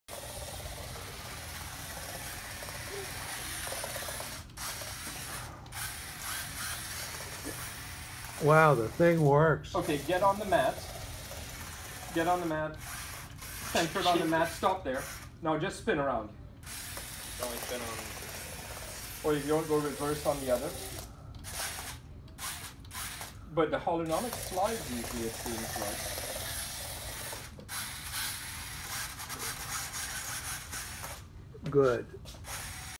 Our Team(#11355) is building our First Tech Challenge Robot
Drive Base is complete